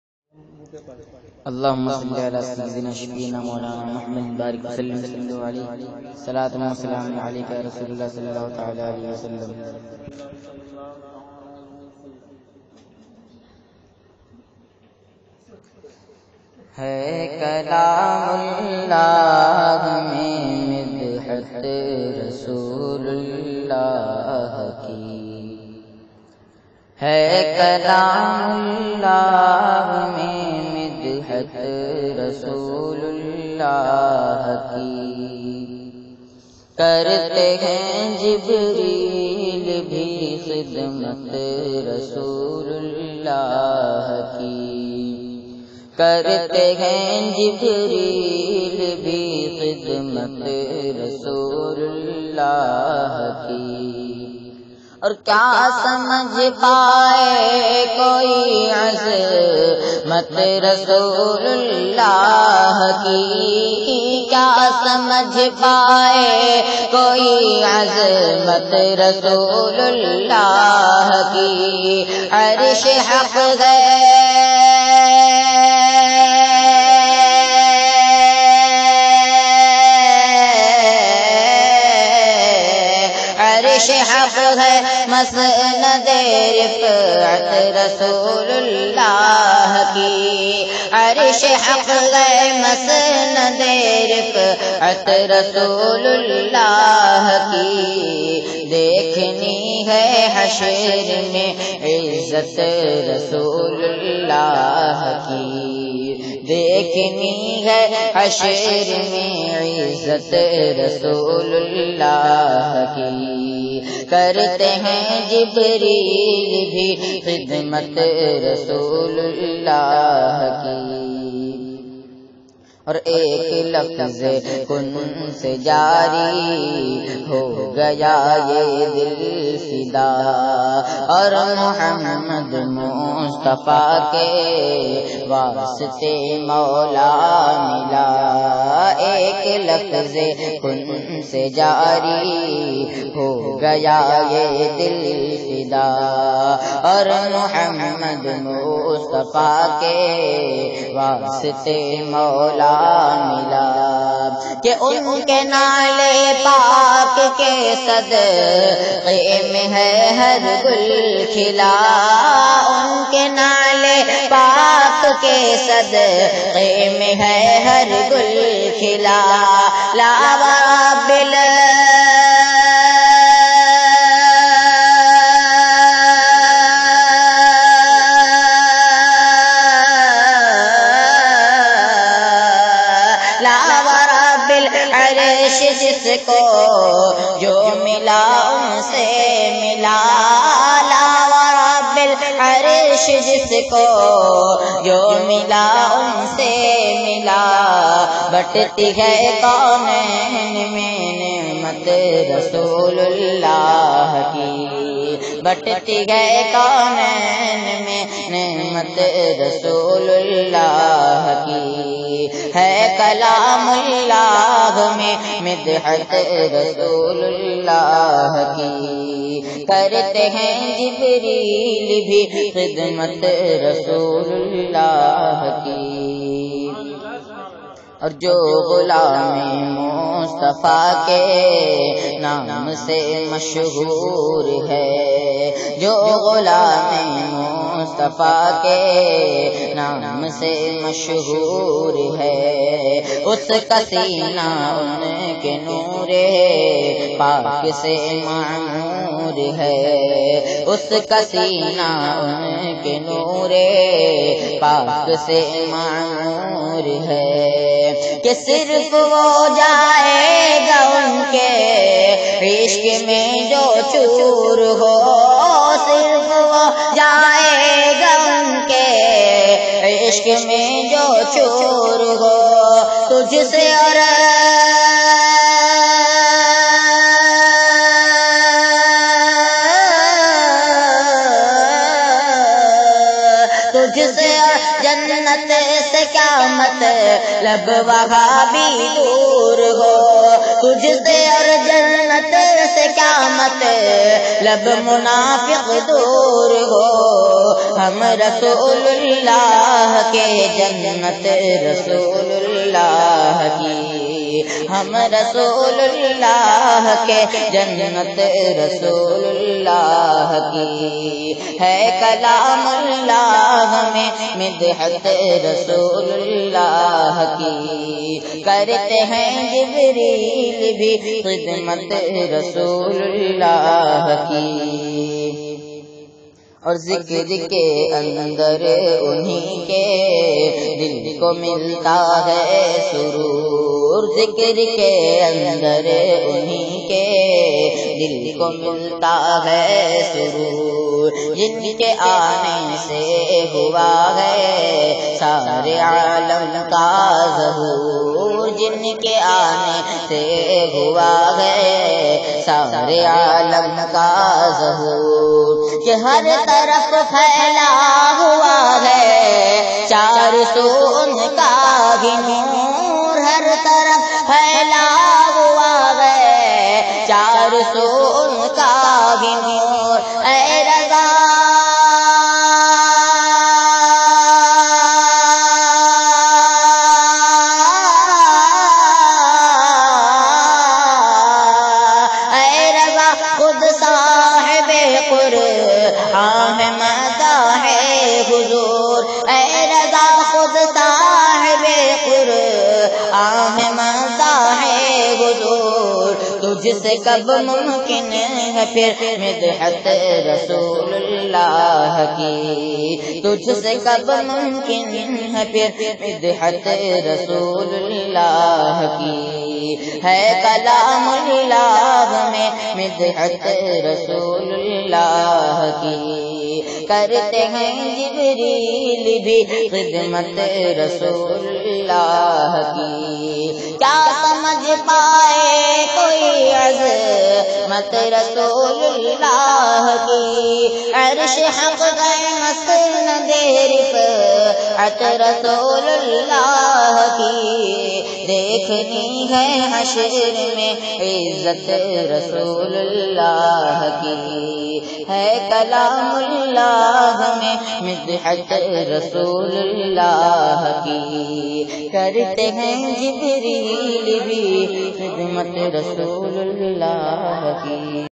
Hafiz mohammed rashid raza_kurla_naat alahazrat
New naat with kalam e alahazrat  hafiz mohammed rashid raza kurla mumbai.